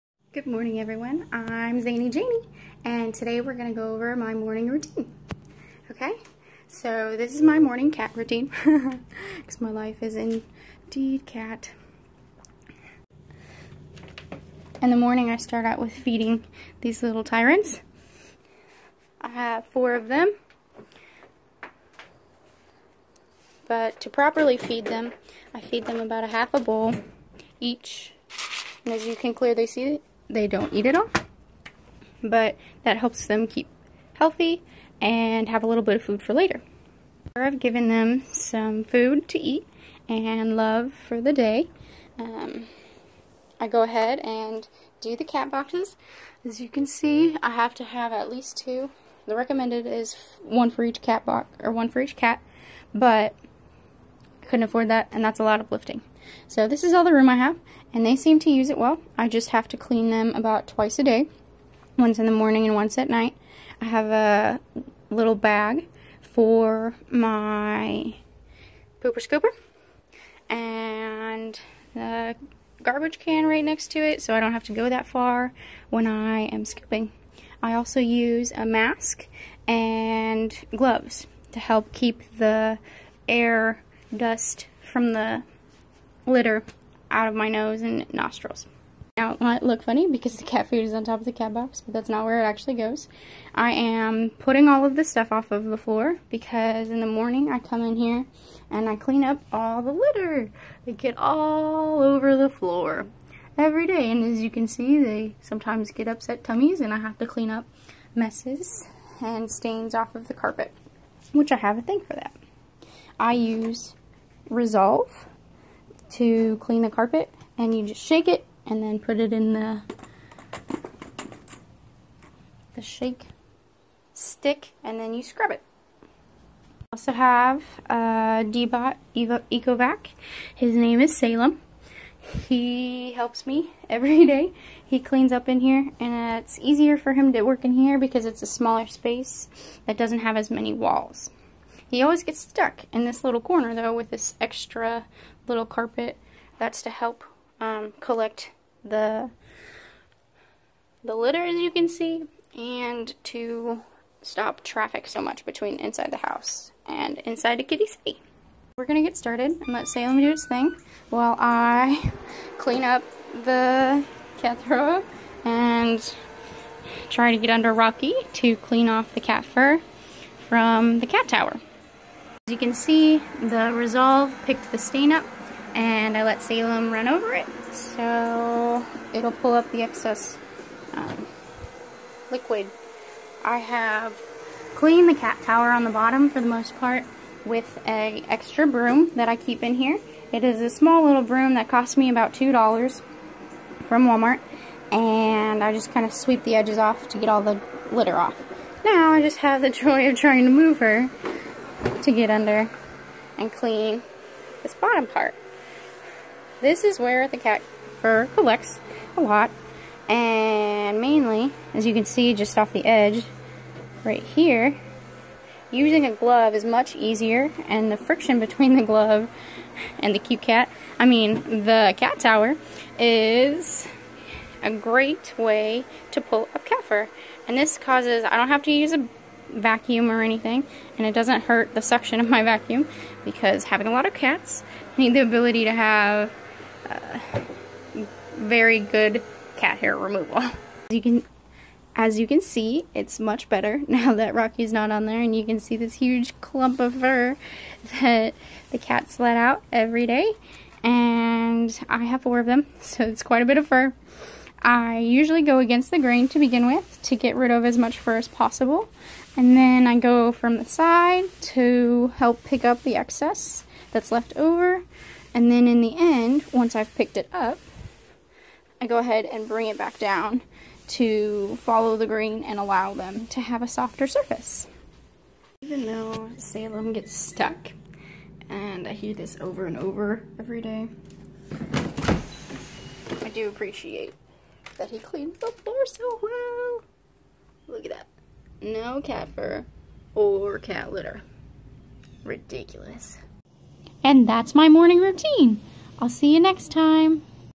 Cat Morning Routine
In this video, I go over how I start my day which begins with my four lovely cats and their messiness!